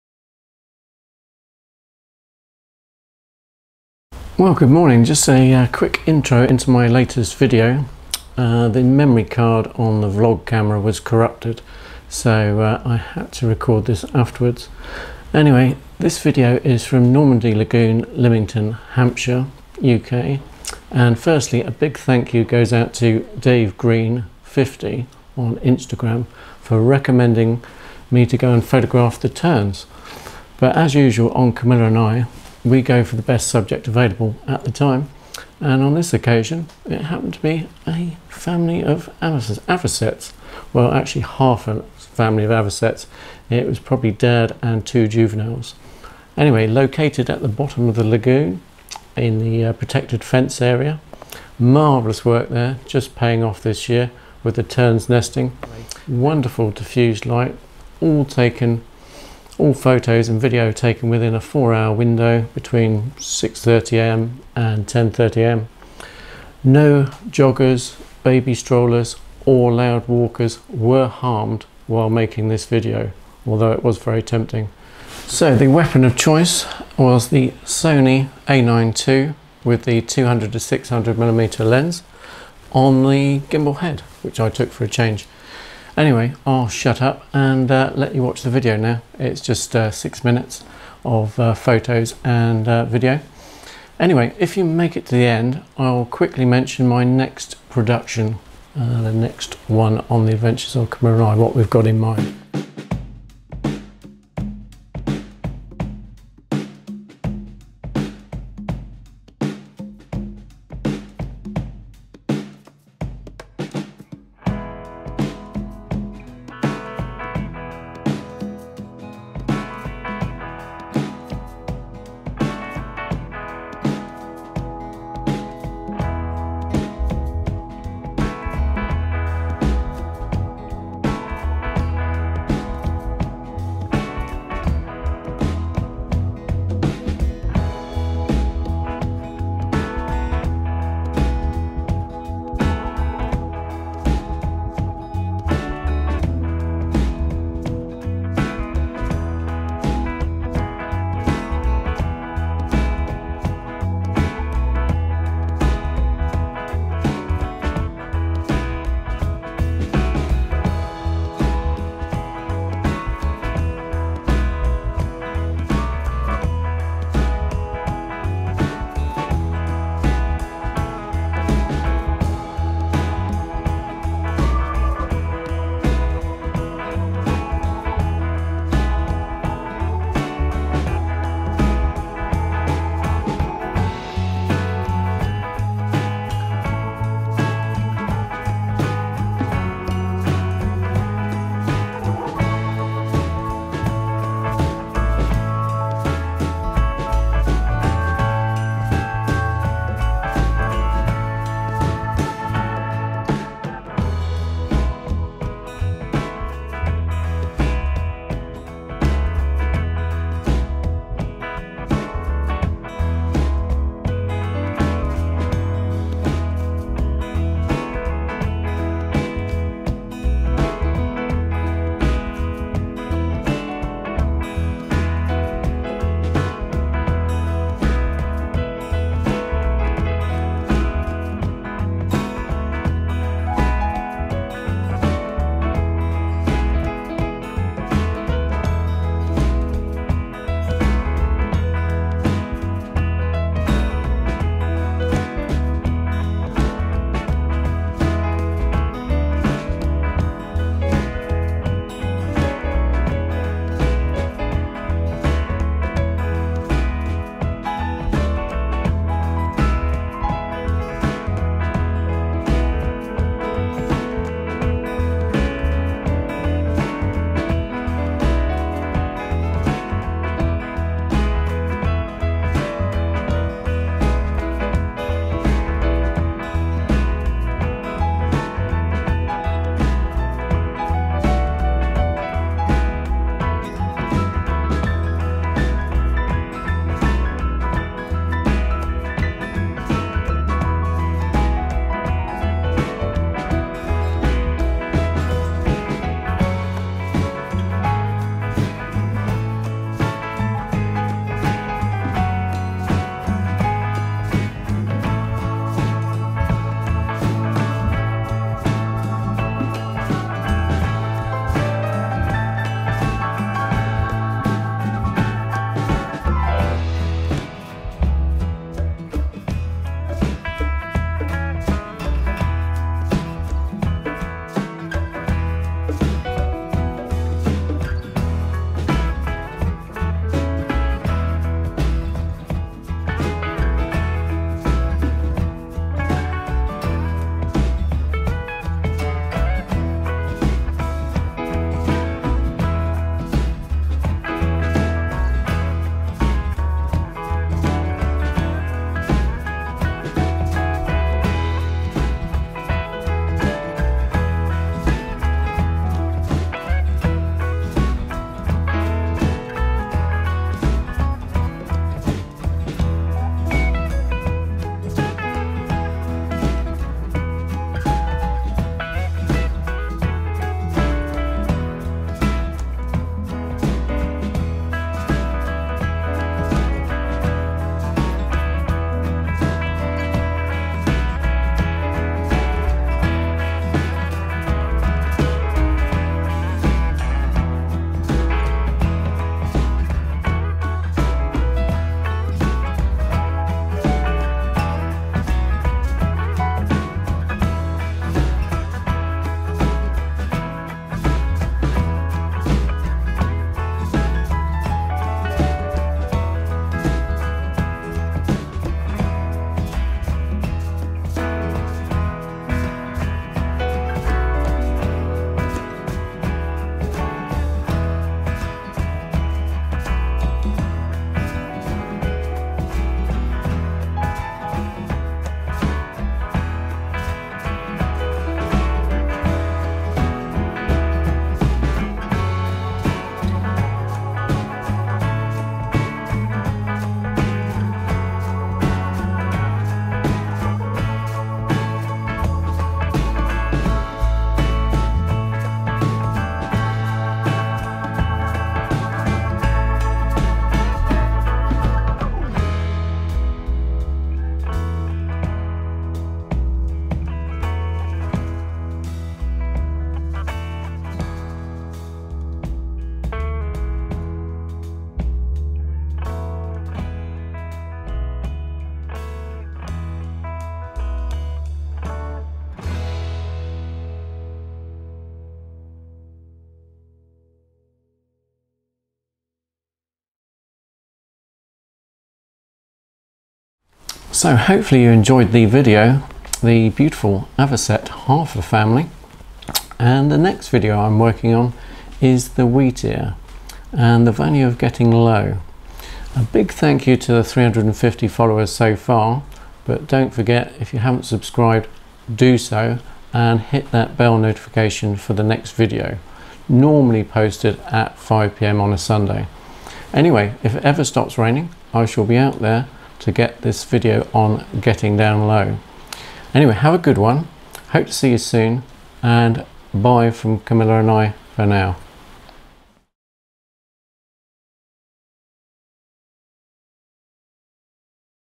Summer wildlife photography of the Avocet wading bird
Summer wildlife photography of the Avocet wading bird.   Wildlife photography with half an Avocet family from Normandy Lagoon Lymington Hampshire UK. Video and pictures of the Avocet taken with the A9II and 200-600mm Sony f5.6 -6.3
Seem to be the Male Avocet having a darker head with two juveniles

A big thanks to dave_green50 on Instagram for directing me to Normandy Lagoon with the intention of photographing the Terns nesting there. But as usual on "Camilla & I" we photograph the best subject at that time hence Avocets!

All taken in a 4 hour spell 6.30am -10.30 am with many dog walkers, cyclists, baby strollers and loud walkers going past with feet of our location a marvellous morning of opportunistic wildlife photography. A six minute video and pics of the Avocet